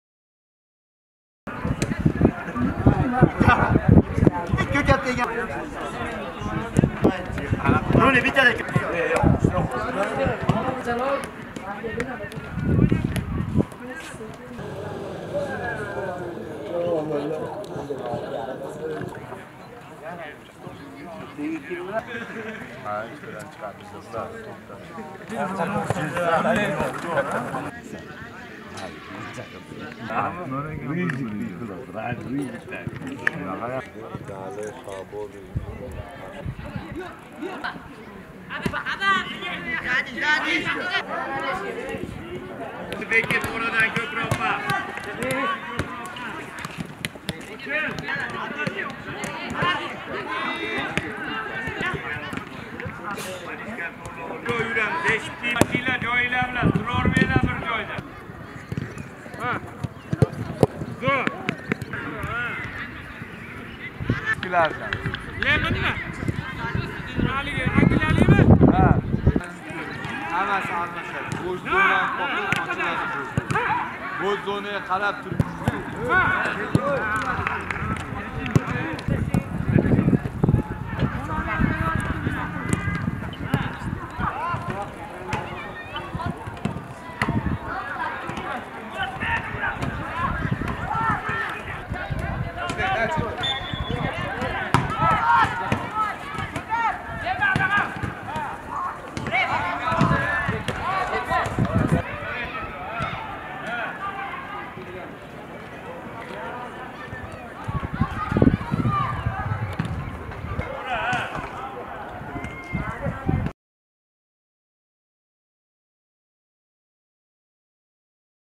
ERTAGA FUTBOL. Jamoamiz "Neftgazmontaj"ga qarshi bahs uchun o'yin oldi mashg'ulotini o'tkazdi
ERTAGA FUTBOL
Birinchi liga-2022
6-tur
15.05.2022
Andijon shahar (Sho'rbuloq)
"Akademiya" stadioni
Soat 17:30
"ANDIJON-SGS" - "NEFTGAZMONTAJ"